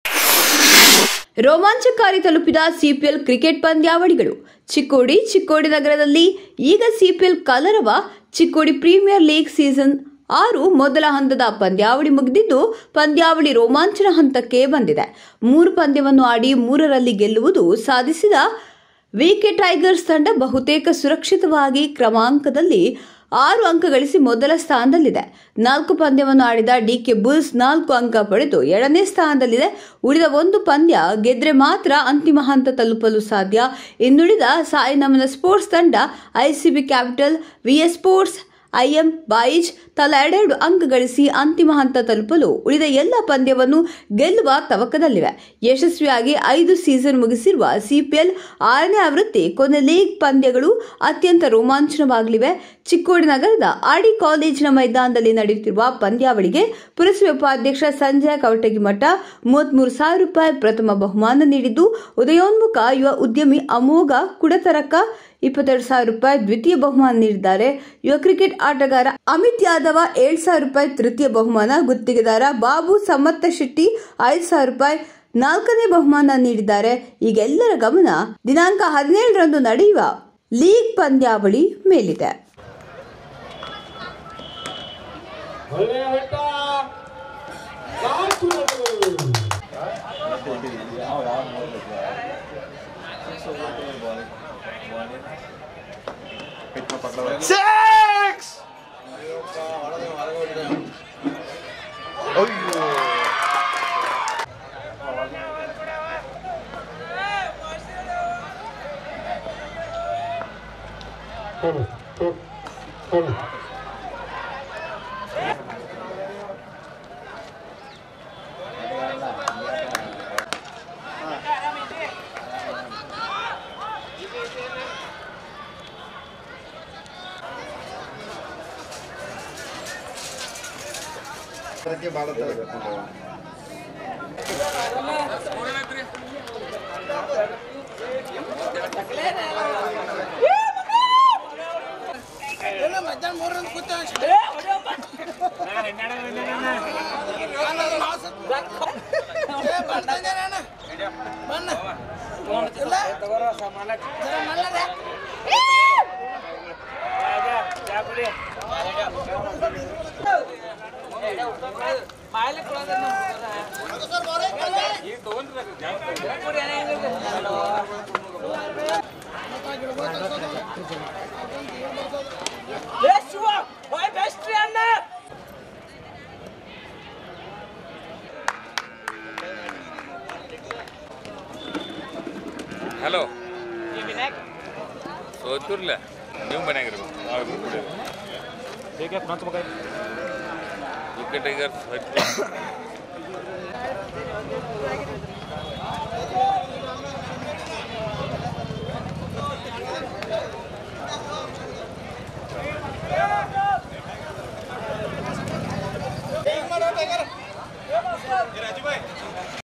ಚಿಕ್ಕೋಡಿ : ರೋಮಾಂಚಕಾರಿ ತಲುಪಿದ ಸಿಪಿಎಲ್ ಪ್ರೀಮಿಯರ್ ಲೀಗ್ ಸೀಸನ್ 6. ಕ್ರಿಕೆಟ್ ಪಂದ್ಯಾವಳಿಗಳು
ಚಿಕ್ಕೋಡಿ : ರೋಮಾಂಚಕಾರಿ ತಲುಪಿದ ಸಿಪಿಎಲ್ ಪ್ರೀಮಿಯರ್ ಲೀಗ್ ಸೀಸನ್ 6. ಕ್ರಿಕೆಟ್ ಪಂದ್ಯಾವಳಿಗಳು (ICB NEWS CHIKODI)